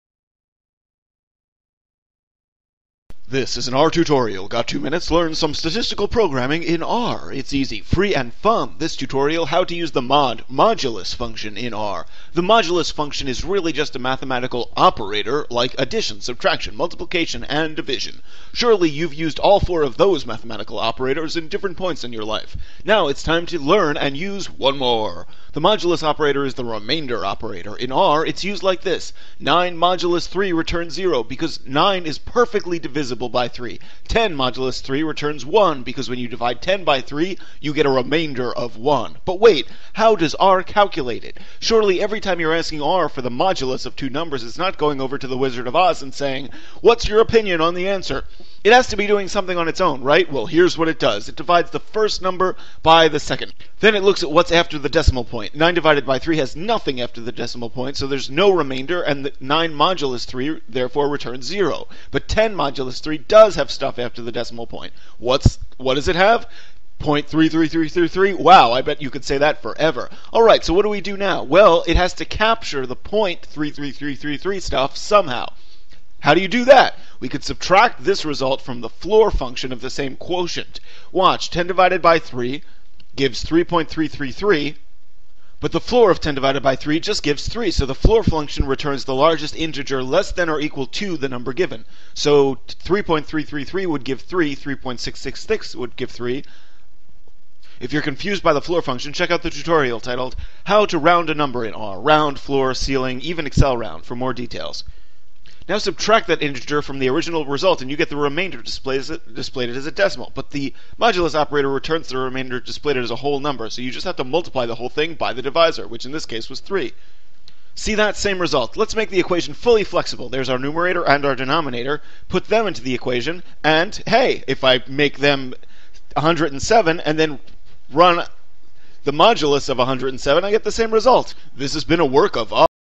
72 how to use the mod modulus) function in r to calculate a remainder
오픈 소스 분석 툴인 R을 소개합니다. 본 동영상은 각각 2분정도의 길이로 R의 기능을 하나씩 알려드립니다.